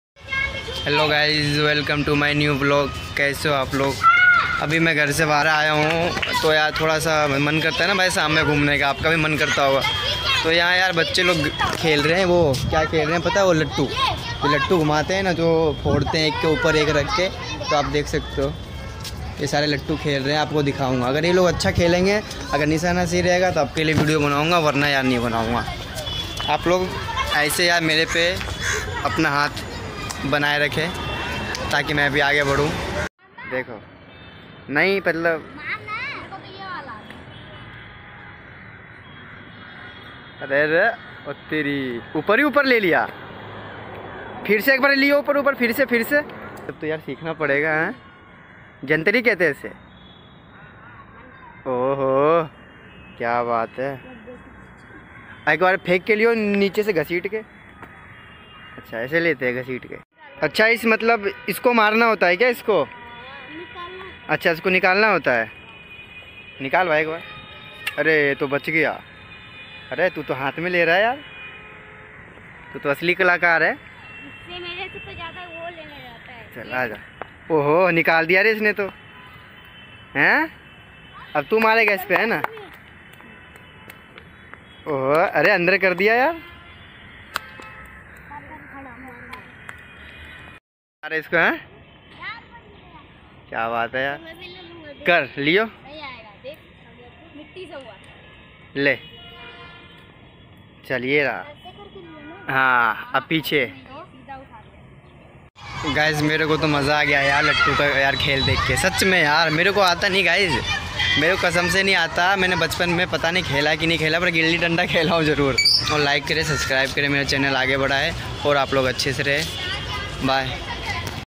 लट्टू नचते नचते टूट गई(Lattu dance crack
Lattu ko nacha ka tor diya ap bhe ya game khela ho agr khela ho to comment kro ap log ya game bachpan ka h maja aa jyga apko